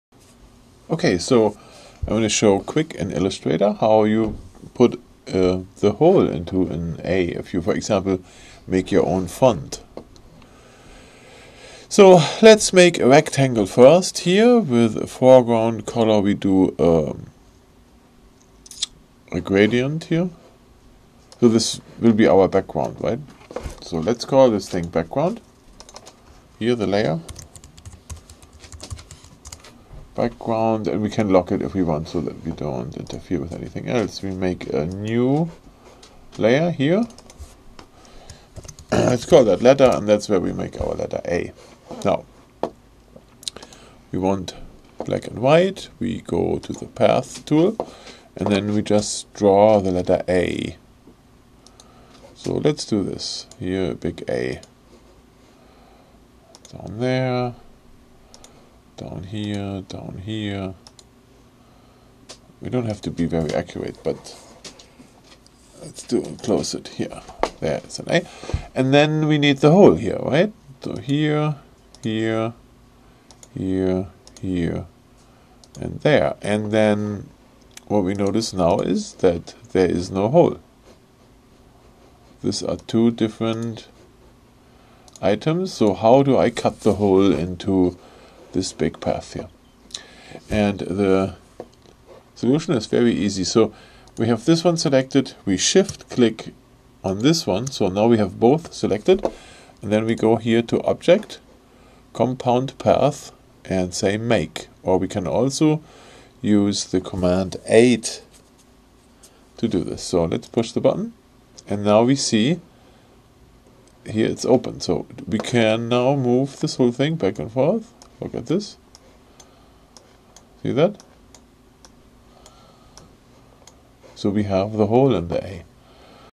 How to make a hole in an A in Adobe Illustrator
Have you ever wondered how that works with the path tool in Adobe Illustrator. How do you get a hole in the A? Is that difficult or not?
No, it's not. Look at this video. In less than 2 minutes you know how it works. Cool stuff, no doubt.